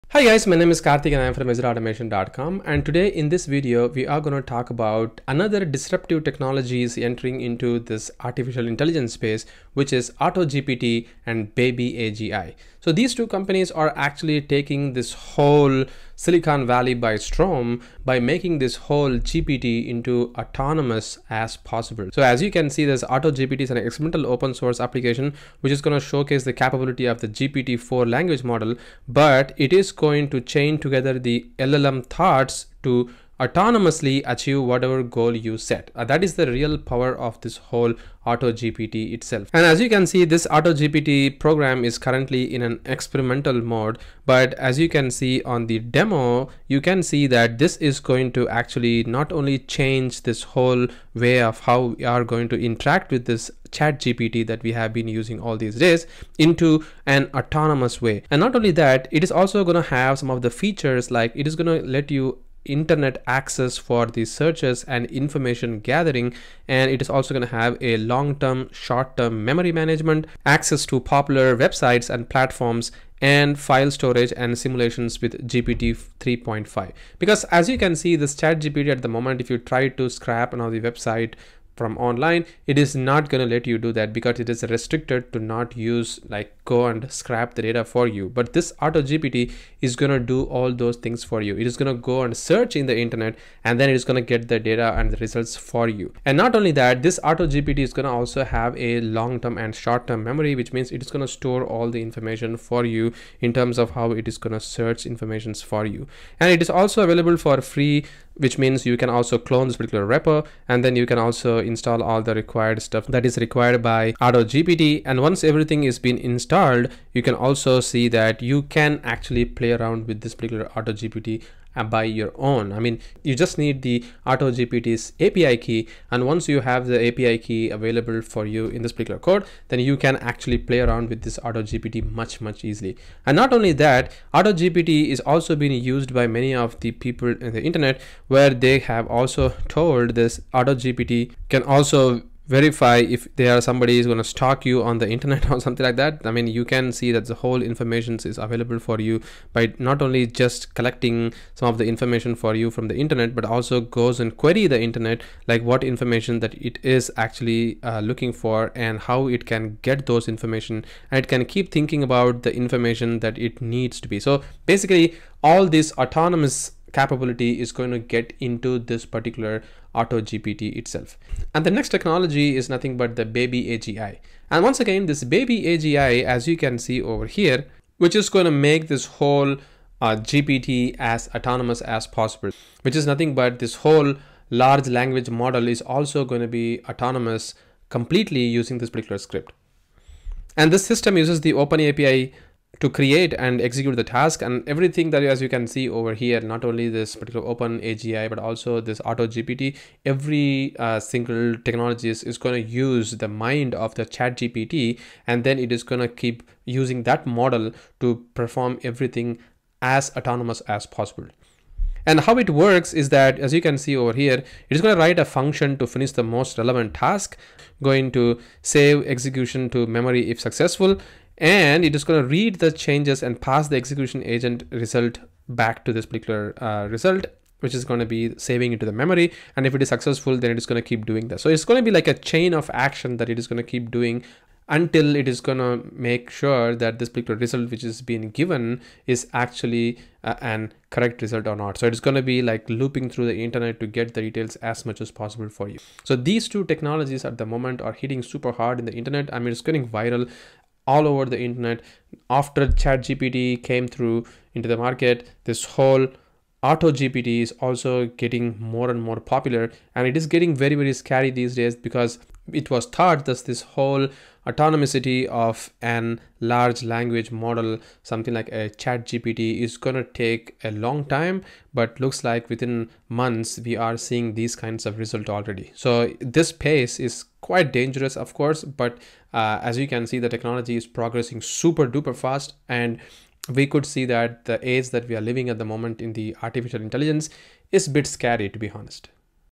AutoGPT and BabyAGI - Autonomous and Self-Improving AI Agents, now that's scary 🤯🤯
Prepare to be blown away once again! While ChatGPT's groundbreaking technology still has you reeling, get ready to welcome the dynamic duo of AutoGPT and BabyAGI. These two self-improving AI agents are ready to autonomously scour the internet and conjure up responses for you on the fly. Get ready to sit back and watch as they work their magic!